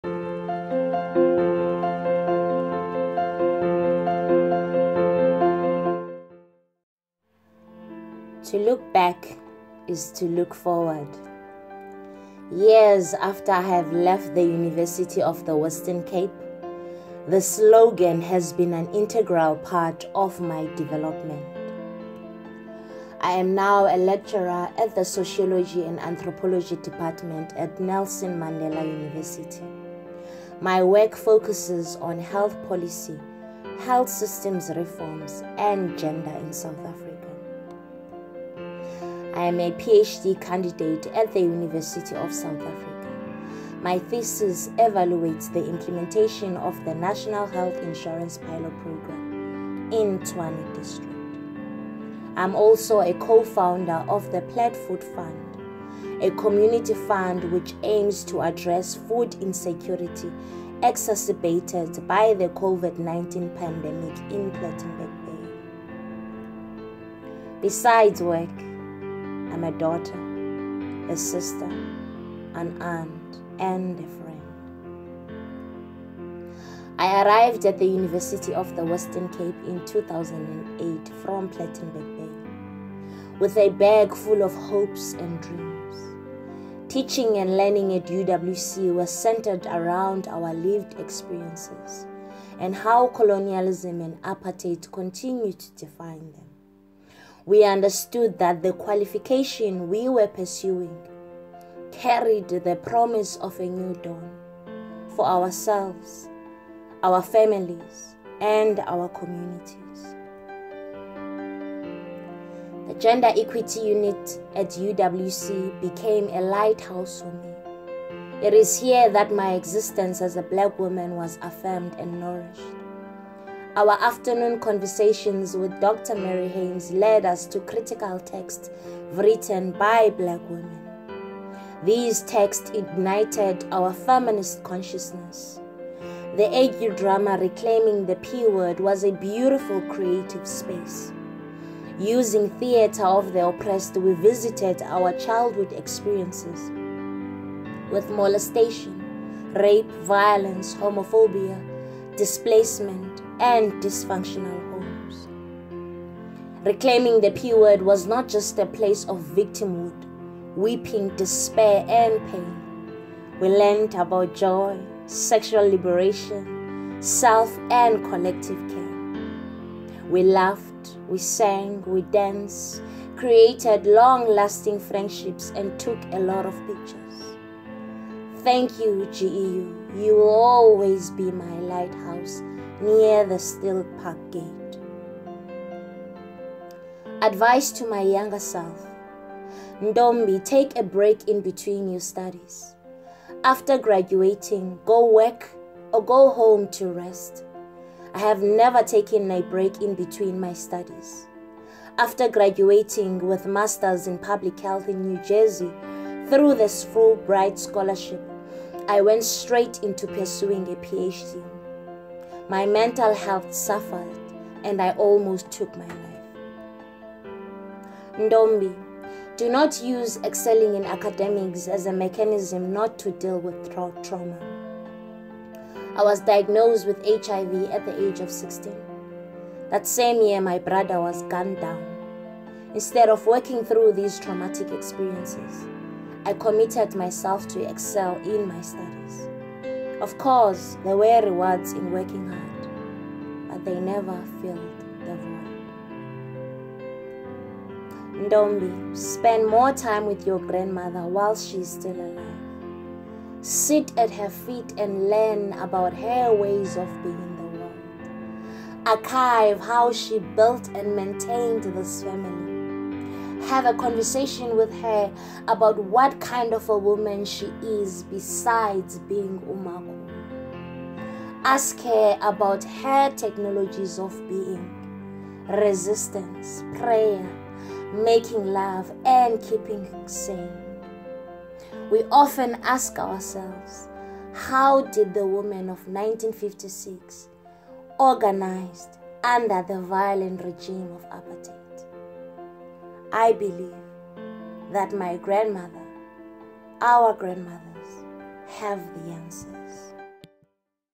UWC 60th Anniversary - Virtual Programme: Ntombi Wonci
Today, the Gender Equity Unit introduces and celebrates Ms Sivuyiswe Wonci. Ms Wonci is an alumna of the University of the Western Cape and a 2013 recipient of the Fullbright Program which enabled her to pursue a Master’s in Public Health (MPH) at Montclair State University in New Jersey, United States. Currently, Ms Wonci is a full-time lecturer in the Department of Anthropology and Sociology at Nelson Mandela University and also works as a researcher at the Institute for Gender Studies at the University of South Africa. Ms Wonci is also currently pursuing her PhD in Sociology at the University of South Africa where her doctoral focus is on the implementation of the National Health Insurance pilot in the Tshwane District. Before this, Ms Wonci worked for the World Health Organisation (WHO), the Centre for Research and Evaluation on Education and Human Services as well as the East Orange Department of Health. She is also the recipient of various international travel and domestic research grants from the National Research Fund (NRF), the Woodrow Wilson National Fellowship Foundation as well as the Mellon Mays Undergraduate Fellowship.

 In addition to the above, Ms Wonci is a performance poet. She primarily writes and performs her poetry in her home language, isiXhosa. As a woman with radical black feminist ideologies, Ms Wonci’s poetry is particularly centred around issues of gender, gender-based violence, social justice, politics, socioeconomic inequalities, HIV/AIDS and mental health disorders. In 2009, one of Ms Wonci’s poems on HIV/AIDS was incorporated into Reclaiming the P… word production by the Gender Equity Unit. In incorporating Ms Wonci’s poem into the production, Reclaiming the P… word became representative of all three official languages of the Western Cape province.